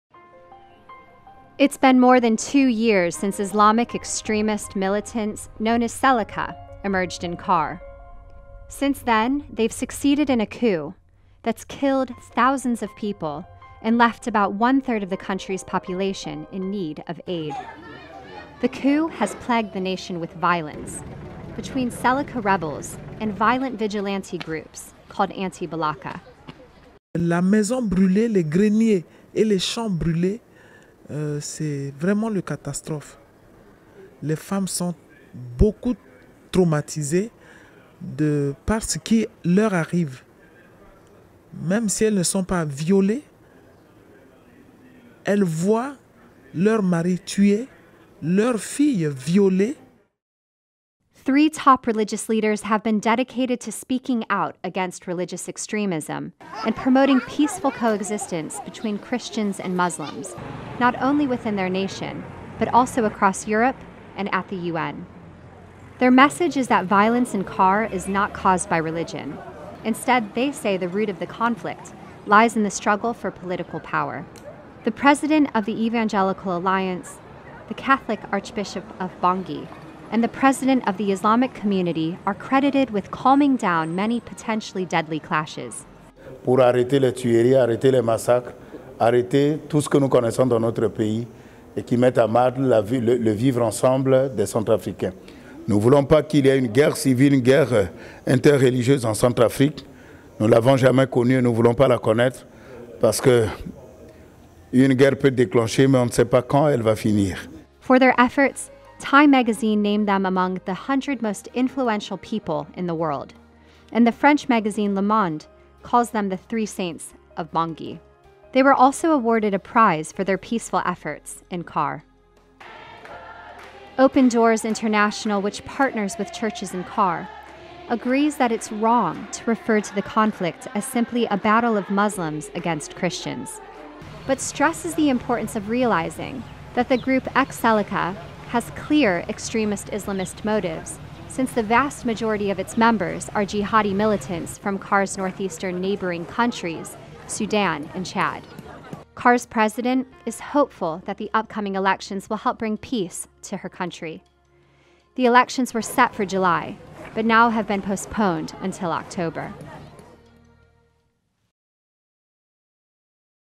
Central African Republic - A forgotten crisis?
On July 24, 2014 rival armed groups in the Central African Republic, CAR, signed a ceasefire agreement aimed at ending an ongoing religious conflict between mainly extremist Islamist Seleka rebels and the vigilante anti-Balaka militia .

Since then the cease-fire pact has been frequently violated but three religious leaders, two Christians and one Muslim, continue to pave the way for peace and reconciliation against the warring elements in CAR.